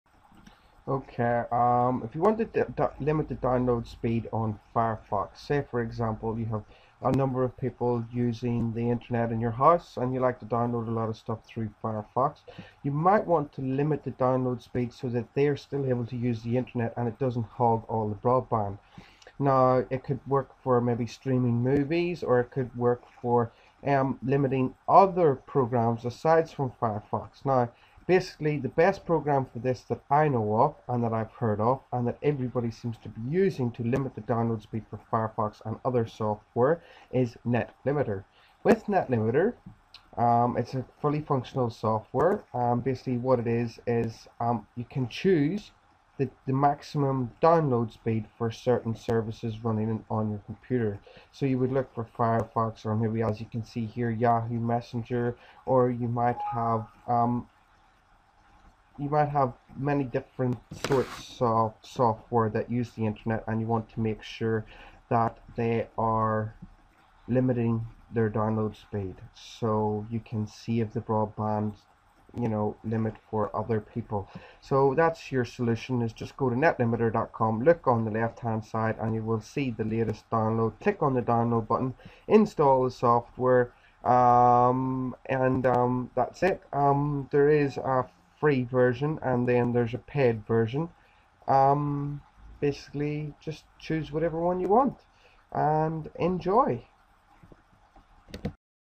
Limit Firefox Download Speed Per Second
A how to for limiting Firefox download limit. So you can stop it ohgging all the bandwidth during downloads.

This solution will also allow you to alter the download speed for any other programs using bandwidth.

It's a freeware solution with a paid option.